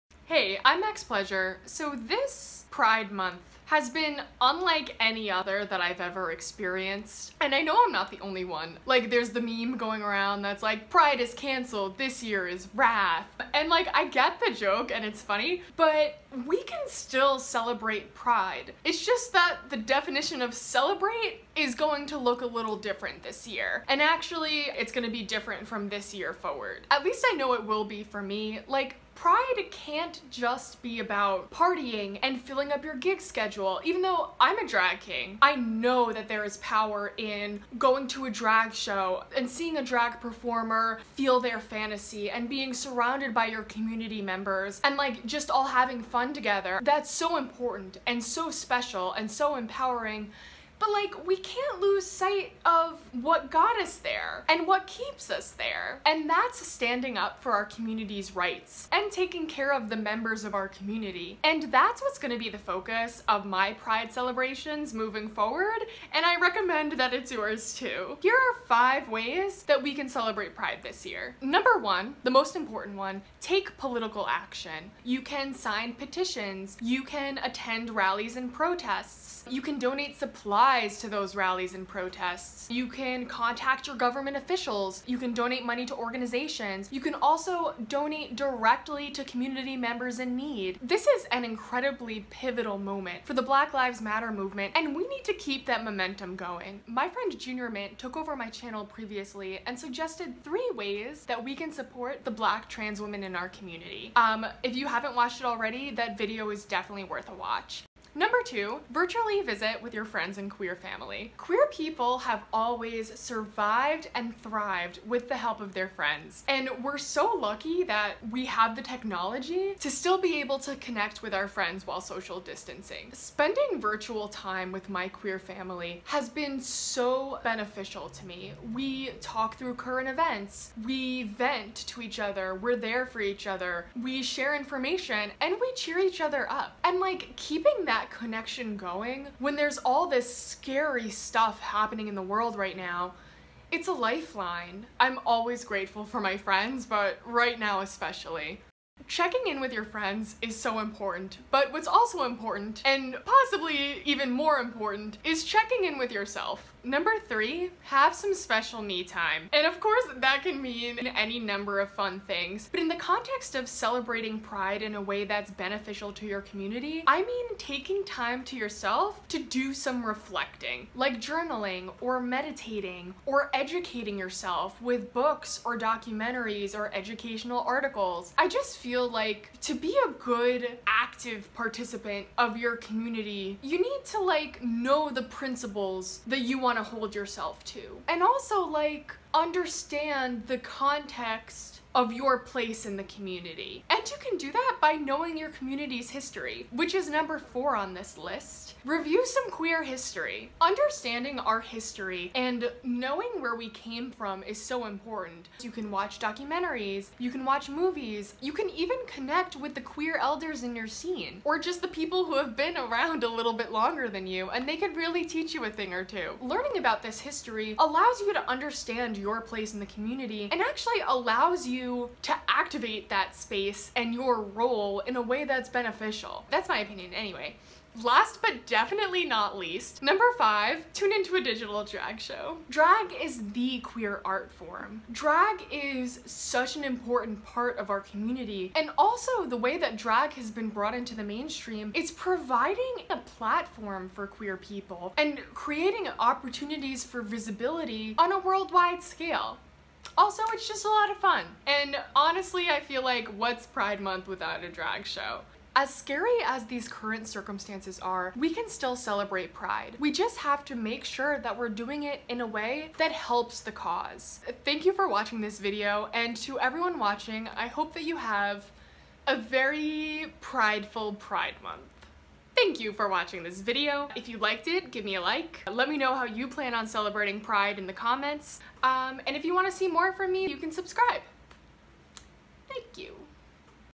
5 Ways We Can Celebrate Pride 2020
NYC Drag King Maxxx Pleasure suggests 5 ways to celebrate pride 2020.

Pride Month 2020 isn't cancelled - it's just going to look very different than previous years. With the COVID19 pandemic and uprising against police brutality in support of Black lives, we have the responsibility to "celebrate pride" virtually/at home in a way that is helpful to our community and in a way that furthers the black lives matter movement.

CREDITS:
Maxxx Pleasure photo by Francine Daveta
Junior Mintt photo by Aleck Venegas
Stonewall image from the Harvard Gazette (Public Domain Dedication)
Pride Flag image by Matt Slocum, AP
Icons made by Freepik and Pixel Perfect from flaticon.com